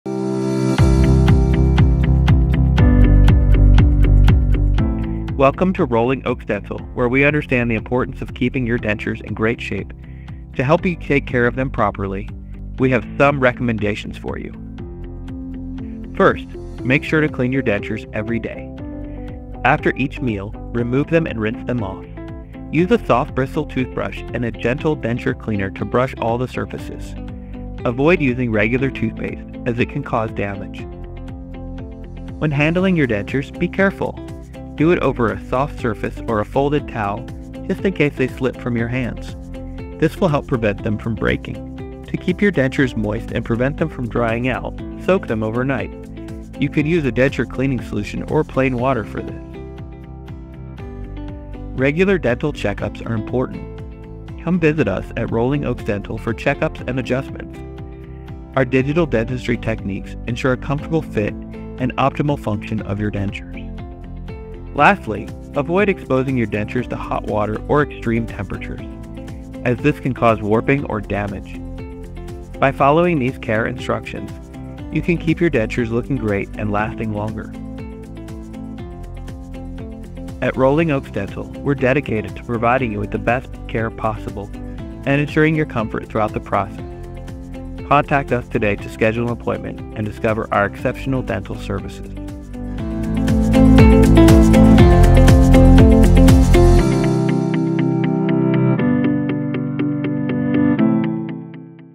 Essential Tips for Maintaining Dentures | Rolling Oaks Dental
At Rolling Oaks Dental in Garden Ridge, Texas, maintaining the longevity of dentures is a top priority. This informative video provides essential tips and specific instructions designed to help patients extend the life of their dentures. It also highlights the advantages of digital dentistry in achieving a comfortable and perfect fit for dentures, ensuring they look and function their best.